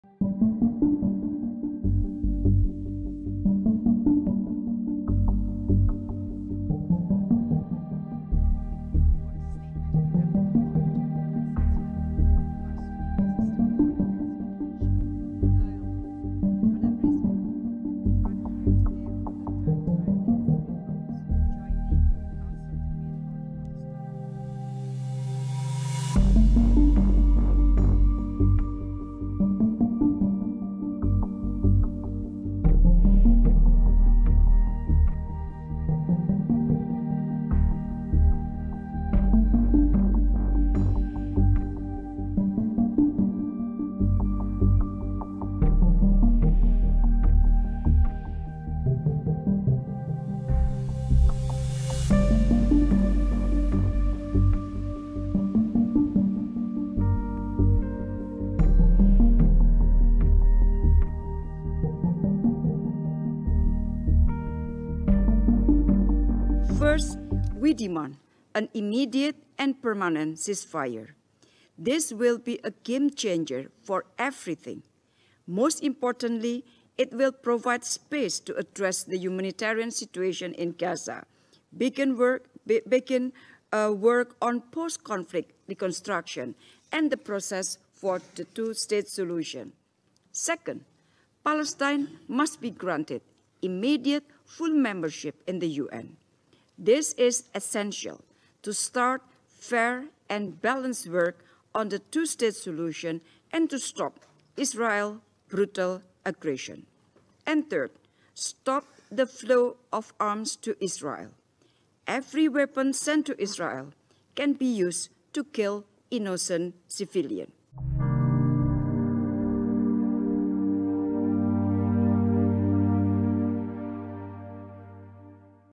Sikap tegas Menlu Retno di debat terbuka DK PBB saat bicara Palestina
ANTARA - Menteri Luar Negeri Retno Marsudi memberikan sikap tegas terhadap kemerdekaan bangsa Palestina di debat terbuka Dewan Keamanan Persatuan Bangsa-Bangsa (DK PBB) di New York, Selasa (23/1) waktu setempat. Menlu Retno juga mengkritik DK PBB yang seolah diam melihat pelanggaran berat Israel dan upaya genosida Israel terhadap Palestina. (Afra Augesti/Sandy Arizona/I Gusti Agung Ayu N/Angel Tabitha)